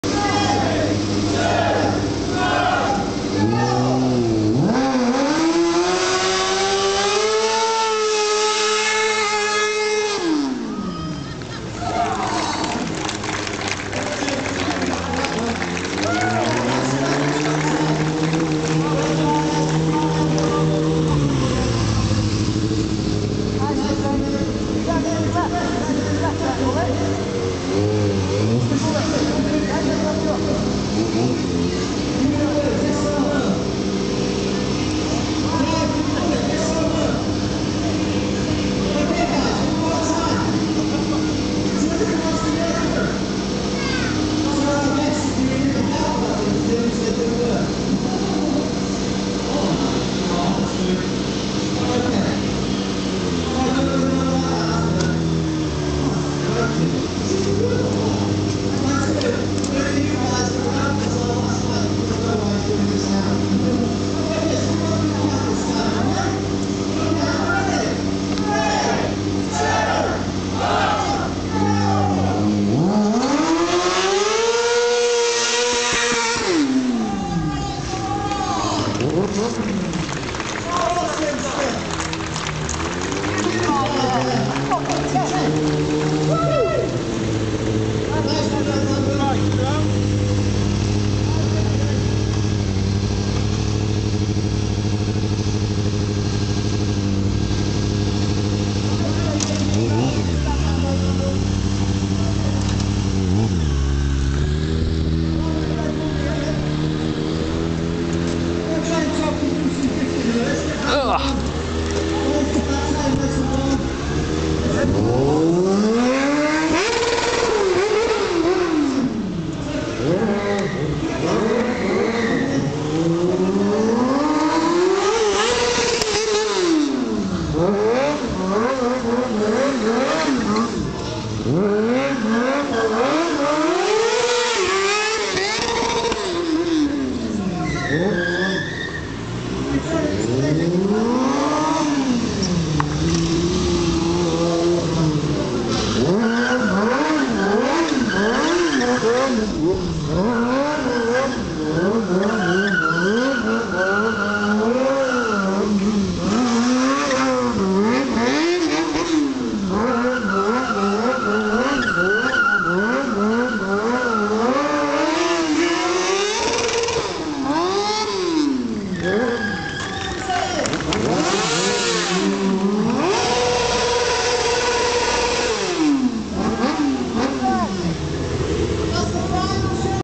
M&P Open Weekend 2013
M&P's 'Spyke' demonstrates the protective properties of covec jeans and jacket in a 'drag test', with the help of 'Two Brothers' stunt display show.